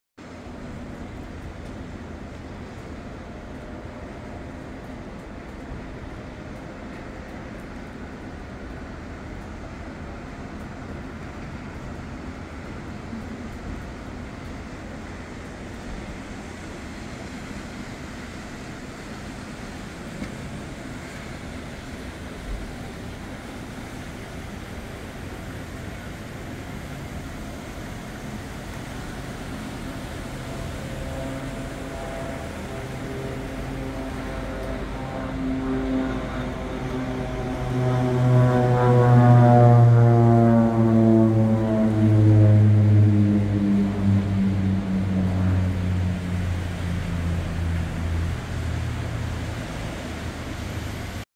Ameriflight Beech C-99 Airliner [N108SX] Takeoff from PDX
Airline: Ameriflight
Aircraft: 1982 Beech C-99 Airliner
Reg: N108SX
Callsign: A81991 | AMF1991
Route: Portland (PDX) - Tillamook (OTK)